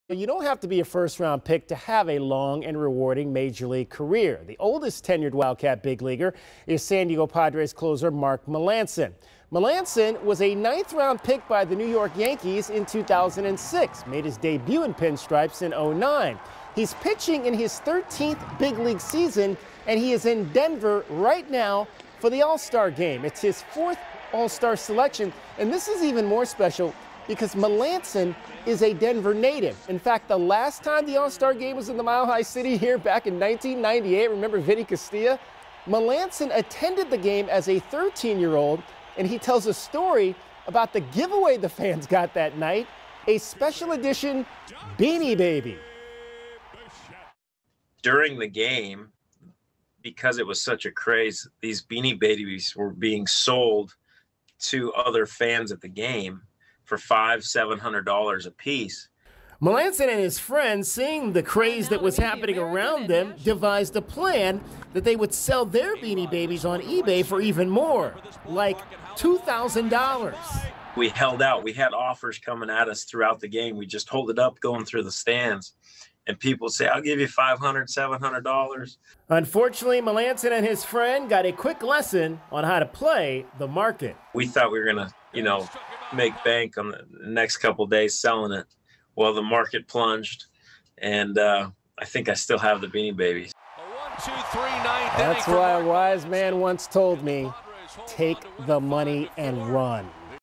Melancon's Beanie Baby scheme
Mark Melancon attended the 1998 Major League Baseball All-Star game in his hometown of Denver, Colorado and he and a friend of his thought they were going to 'get rich quick' on the game's giveaway item to fans.